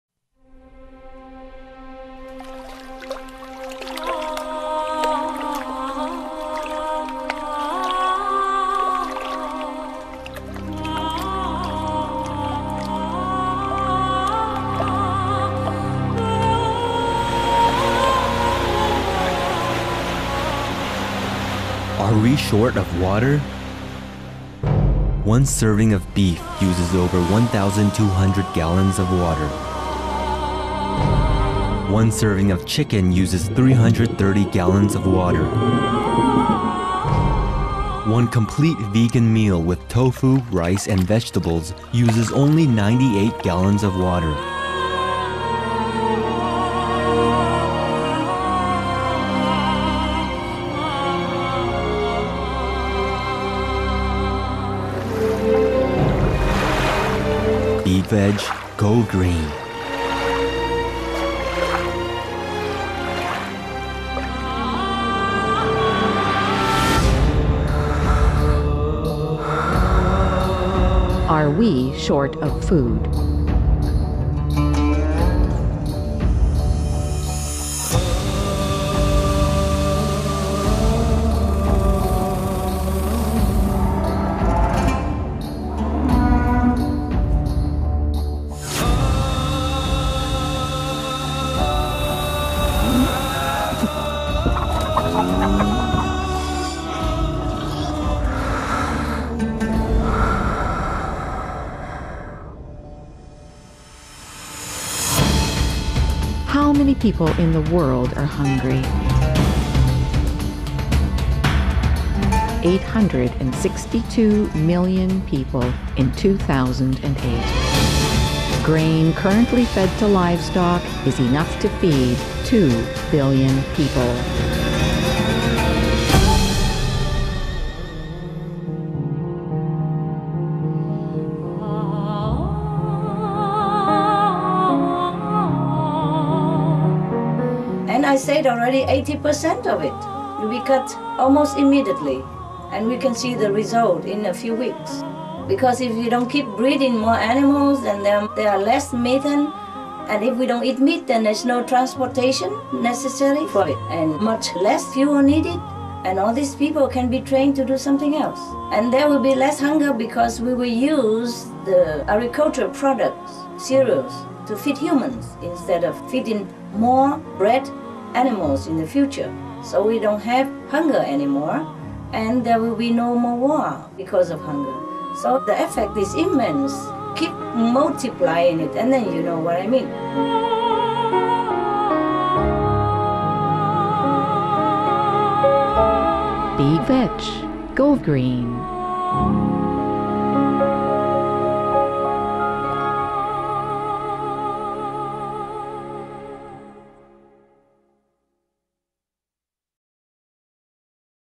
WHY THERE IS A CRISIS?
Very scientific, logical, this video is for anyone that is looking to give others the chance to know the urgent situation related to climate change and also for people to be more sensitive about animal and human suffering. Be Veg, Go Green and Save the Planet! Se Vegetariano, Hazte Ecologico y Salva el Planeta!